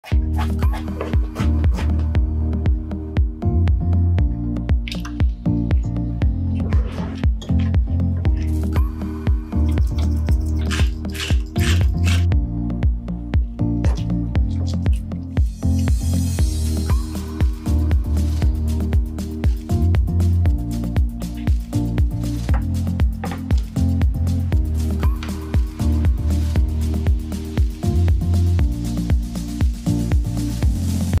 I cook every weekend! Delicious broccoli!
1 broccoli.
Fill with hot water.
Leave on for 10 minutes.
Cheddar Cheese 110g / 3.88oz.
Mozzarella cheese 110 g.
Add two types of cheese.
1 egg.
Breadcrumbs 4 tablespoons.
Salt. Black pepper.
Italian herbs.
Roll in breadcrumbs.
Bake in the oven at 200°C (400°F) for 35-40 minutes.
Greek yogurt/sour cream 1 tablespoon.
Mayonnaise 1 tablespoon.
1 garlic.
Dill.
Juice of 1/2 lemon.
Black pepper.
Bon appetit!